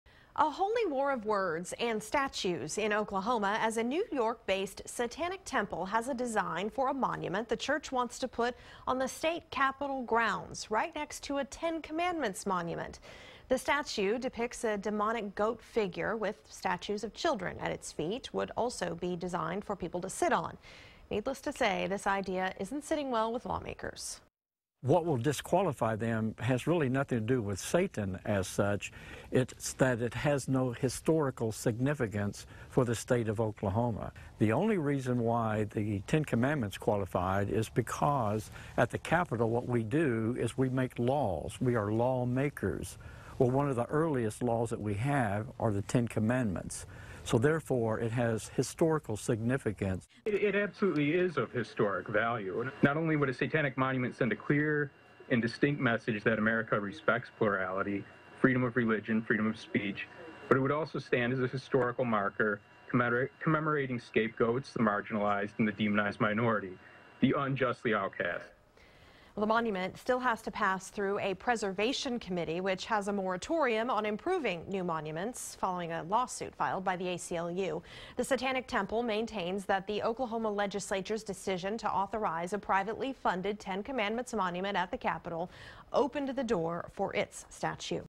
Group unveils Satan statue design for Oklahoma State Capitol
A satanic group unveiled designs Monday for a 7-foot-tall statue of Satan it wants to put at the Oklahoma state Capitol, where a Ten Commandments monument was placed in 2012.